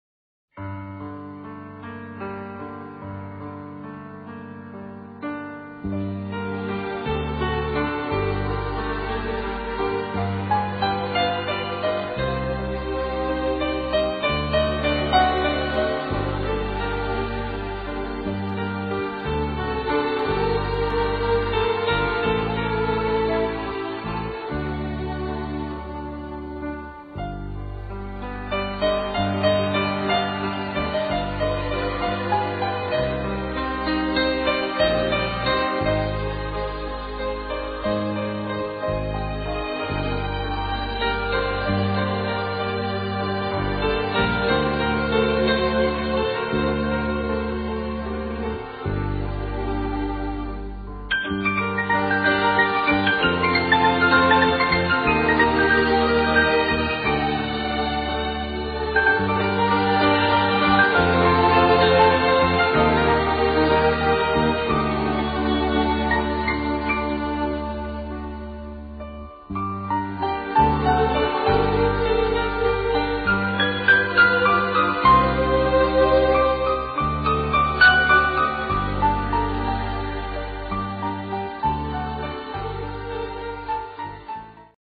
Tibetan Mastiff puppy 5 month Hong Yueliang the Odysseus en his friend the cat Jerry
Yueliang and Jerry there are the biggest friends